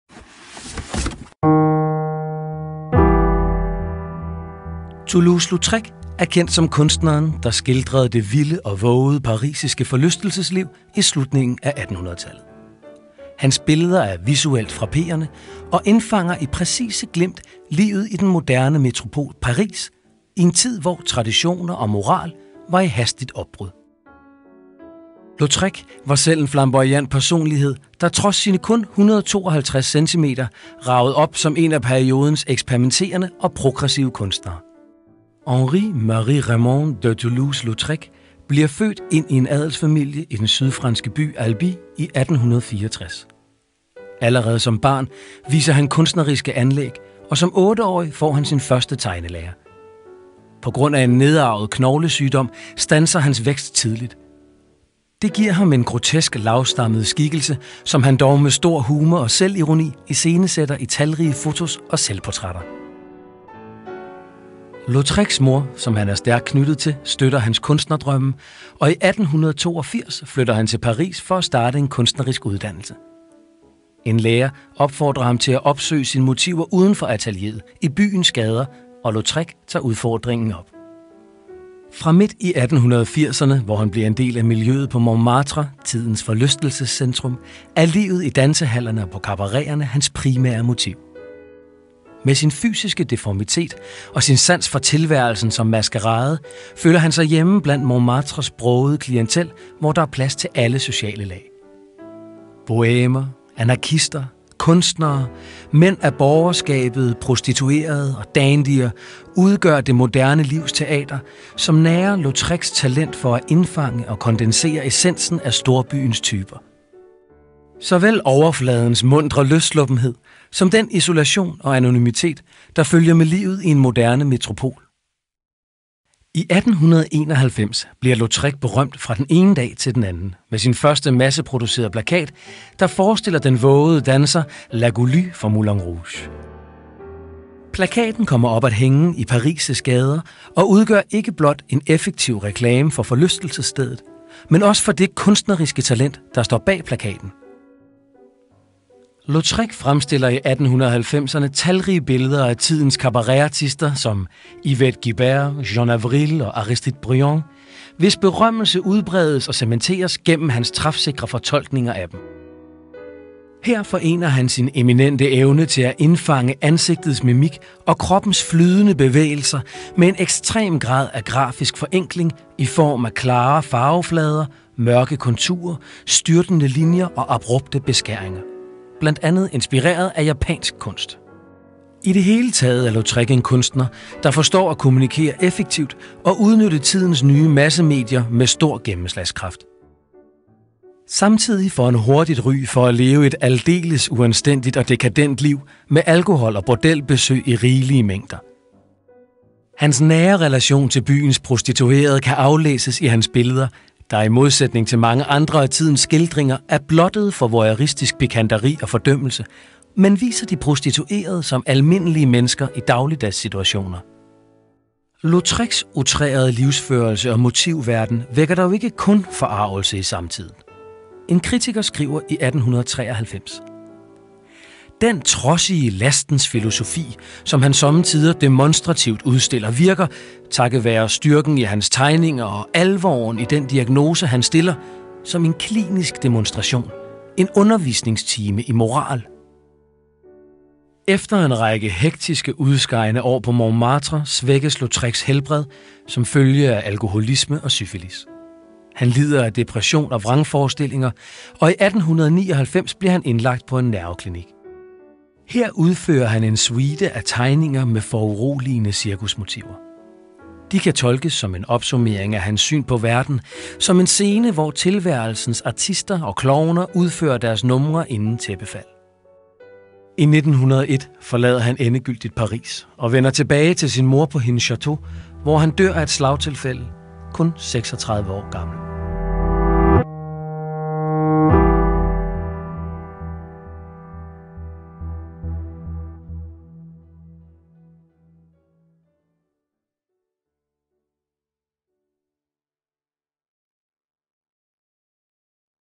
Toulouse Lautrec - Portrait
An insight to the life and works of artist Toulouse Lautrec.